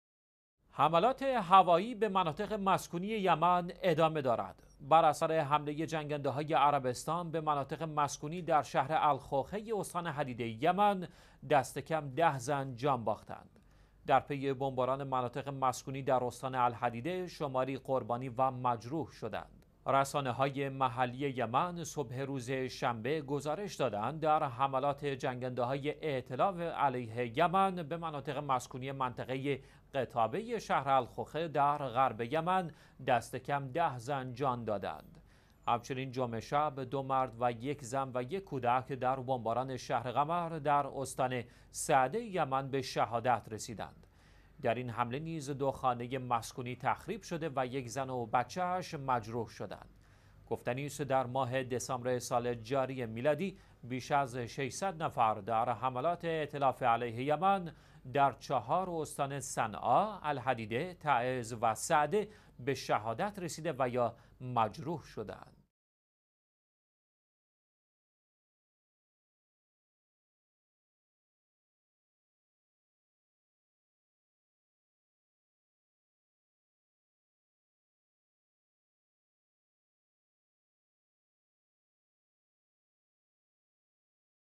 بمباران مناطق مسکونی استان حدیده یمن
حملات هوایی به مناطق مسکونی یمن ادامه دارد.

بر اثر حمله جنگنده‌های عربستان به مناطق مسکونی در شهر الخوخه استان الحدیده یمن، دست‌کم ۱۰ زن جان باختند.

در پی بمباران مناطق مسکونی در استان الحدیده شماری قربانی و مجروح شدند.

رسانه‌های محلی یمن صبح امروز شنبه گزارش دادند در حملات جنگنده‌های ائتلاف علیه یمن، به مناطق مسکونی منطقه قطابه شهر الخوخه در غرب یمن دست کم ۱۰ زن جان دادند.

همچنین جمعه شب دو مرد و یک زن و یک کودک در بمباران شهر غمر در استان صعده یمن به شهادت رسیدند.

در این حمله نیز دو خانه مسکونی تخریب شده و یک زن و بچه اش مجروح شدند.

گفتنی است در ماه دسامبر سال جاری میلادی بیش از ۶۰۰ نفر در حملات ائتلاف علیه یمن در چهار استان صنعاء، الحدیده، تعز و صعده به شهادت رسیده و یا مجروح شدند.

الأحد - ١٢ ربيع الثاني ١٤٣٩